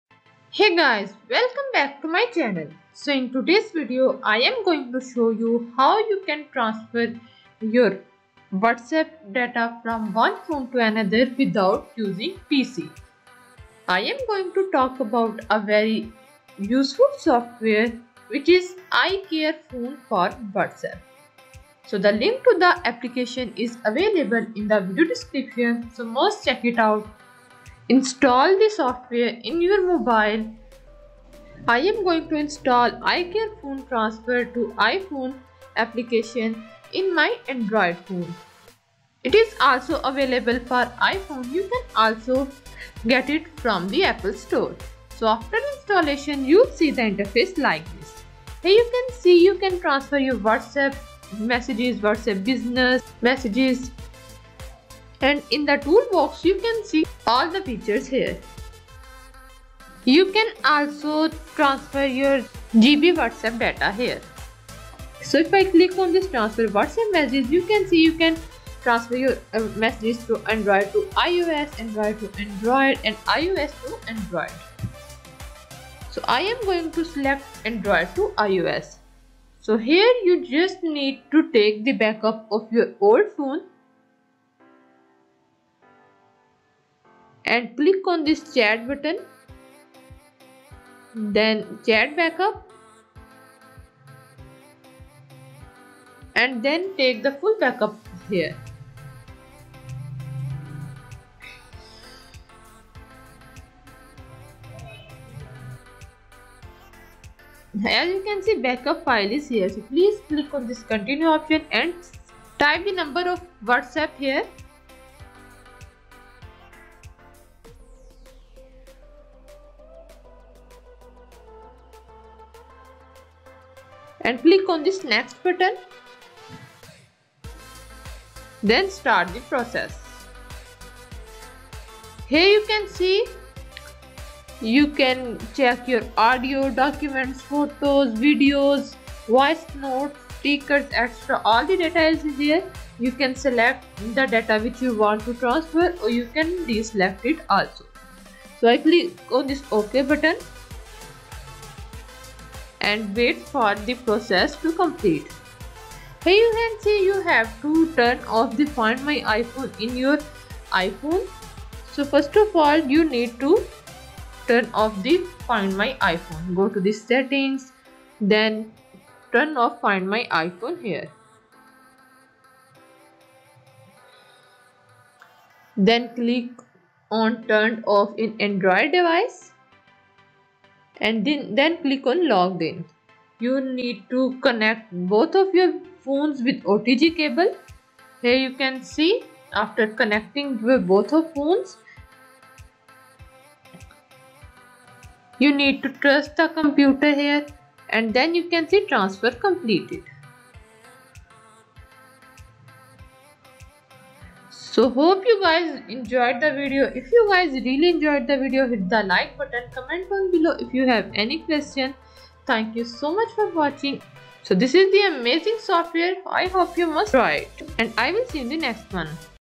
【No PC】Transfer WhatsApp Messages from Android to iPhone 15 in 2024! EASY and SAFE!!
Transfer WhatsApp between Android and iPhone
Transfer WhatsApp/WhatsApp business between Android and iOS with one click.